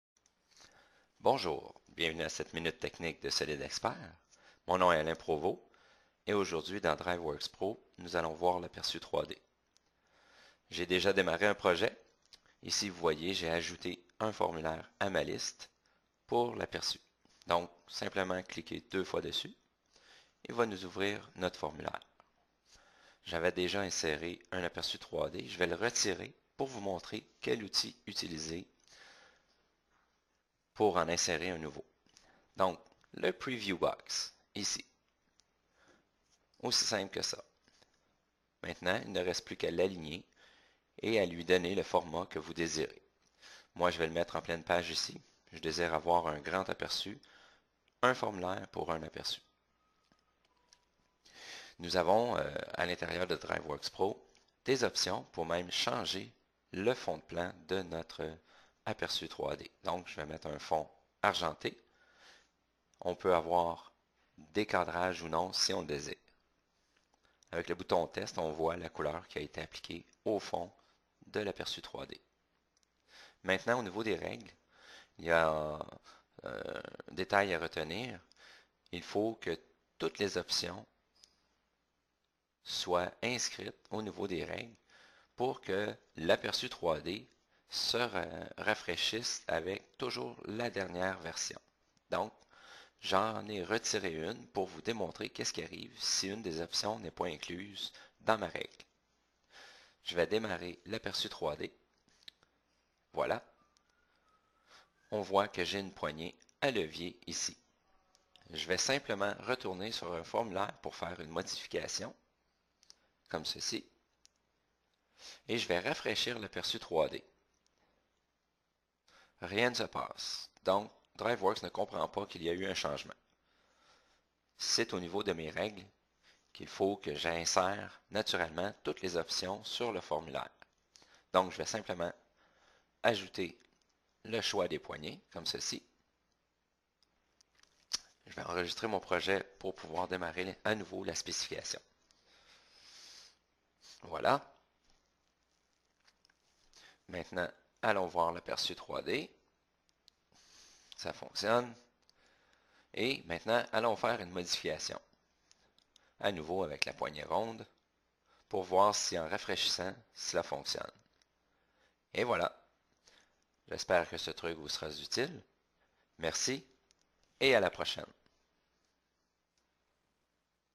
DriveWorks Truc Technique - Aperçu 3D - Avril 2016
Avant de finaliser un projet, il est intéressant d’avoir l’aperçu de notre modèle selon les options choisies mais en 3D. Une image fixe n’est pas suffisant parfois, c’est pourquoi avec Driveworks il est possible d’avoir un aperçu de notre modèle en 3D. Venez voir comment faire.